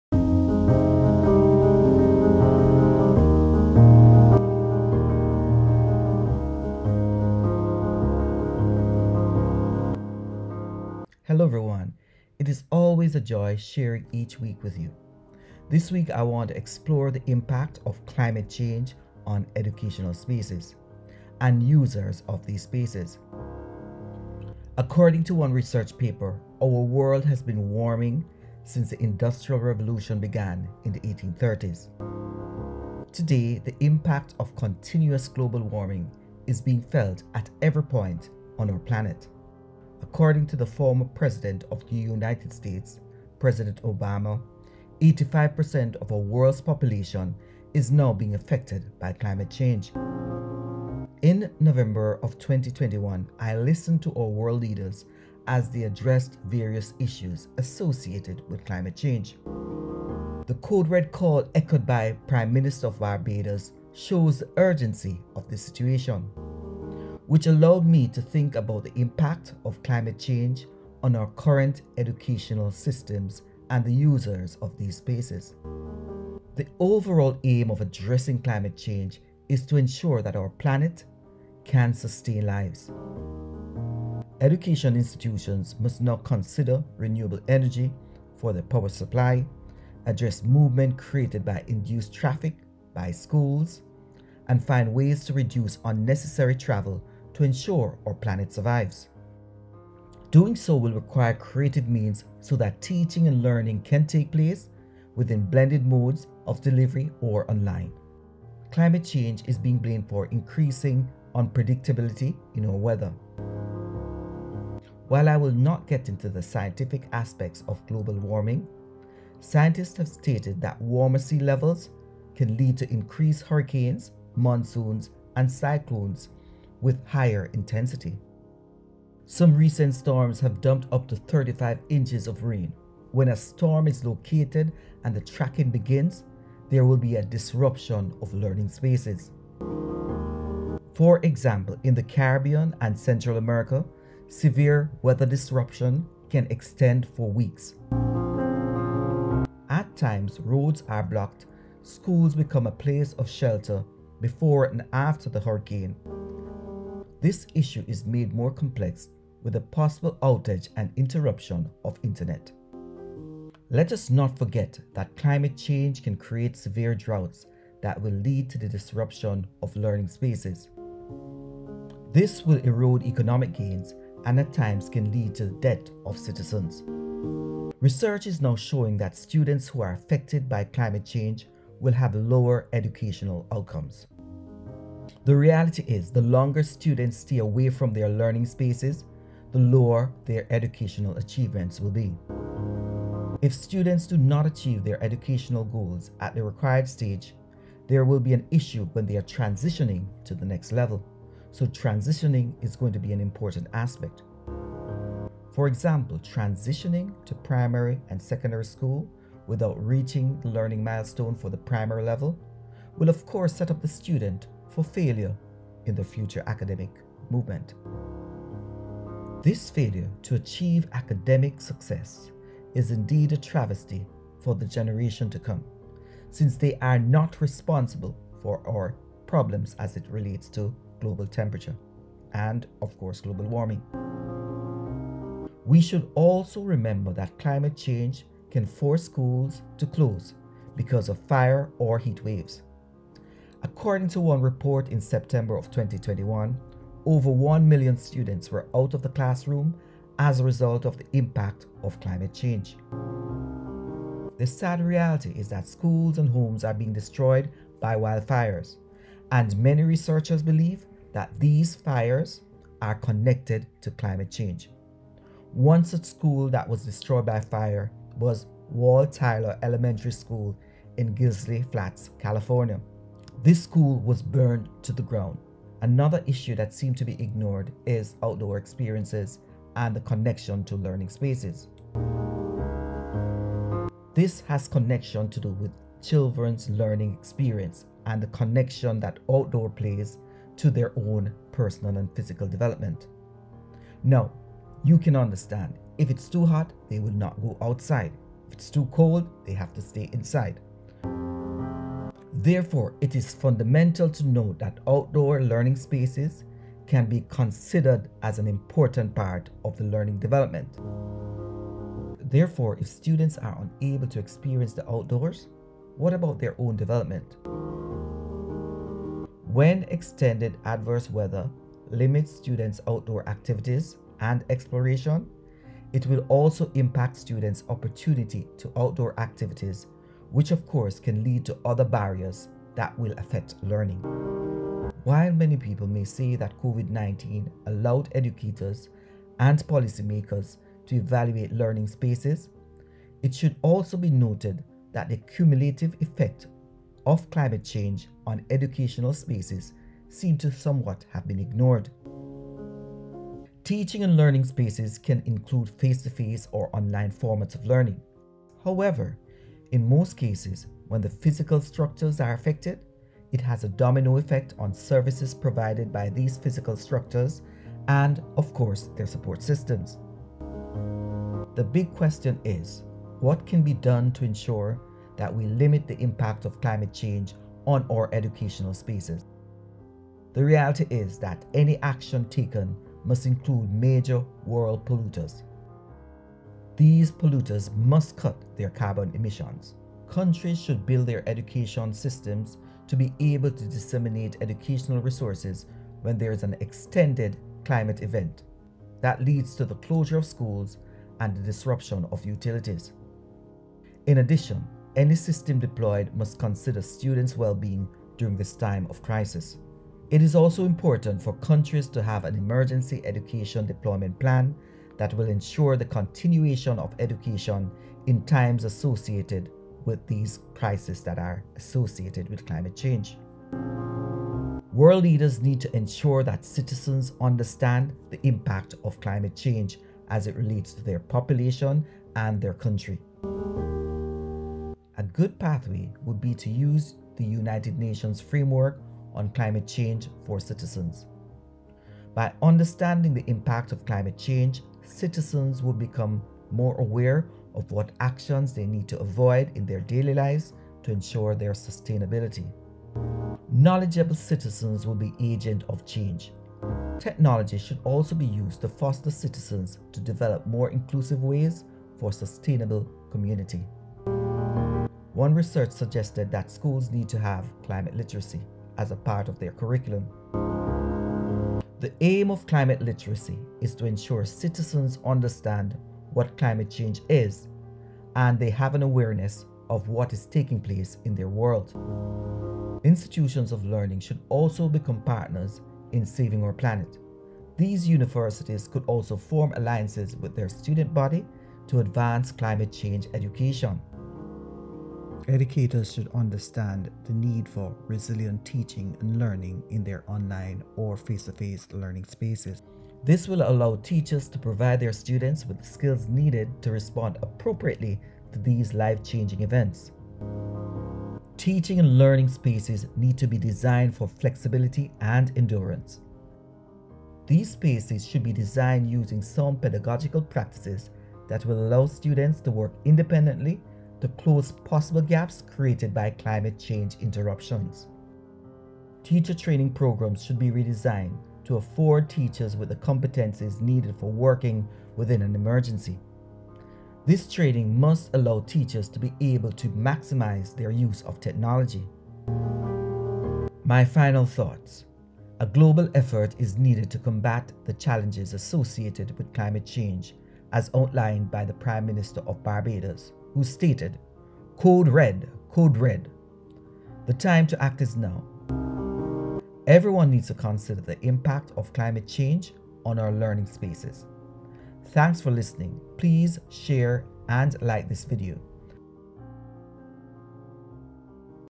THE IMPACT OF CLIMATE CHANGE ON LEARNING SPACES AND LEARNING OUTCOMES
Dr. Jowallah examines the impact of climate change on learning spaces and learning outcomes. This reflective thought provoking presentation should be shared with others. @OurChangingClimate @unitednations